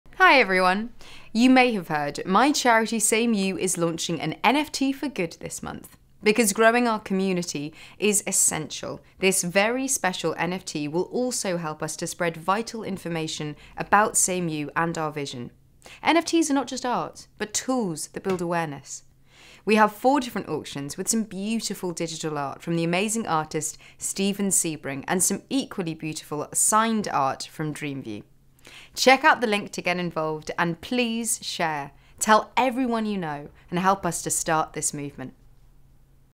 SameYou's NFT for Good in partnership with Seva.Love
Big news! SameYou org has launched our very own NFT for Good to support brain injury recovery.

We are so grateful to our partners who have made this happen. SEVA.LOVE is pioneering nftsforgood and lending their support to help the brain injury community by partnering with us for their first groundbreaking drop.